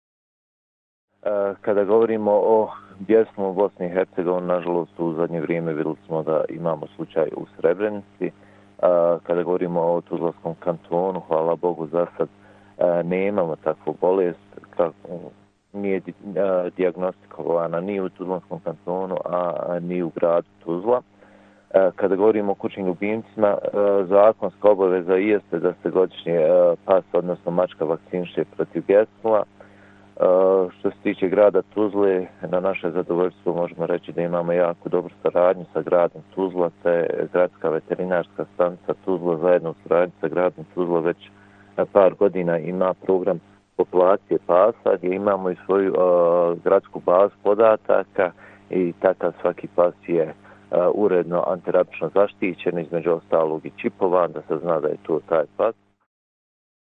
U Tuzli nema bjesnila među životinjama - 12.06.2020.
Nacionalna referentna laboratorija za bjesnilo Bosne i Hercegovine je krajem prošlog mjeseca dijagnosticirala bjesnilo kod leša lovačkog psa s područja općine Srebrenica, što je zadobilo pažnju javnosti i sve pozvalo na oprez. Bjesnilo je zoonaza, virusna bolest koja se prenosi putem pljuvačke sa životinja na čovjeka i mogu oboljeti svi sisari. Ukoliko se adekvatno ne liječi može imati fatalne posljedice. Redovna vakcinacija pasa i mačaka prvi je korak u prevenciji širenja bjesnila. Na području TK nema registrovanih slučajeva.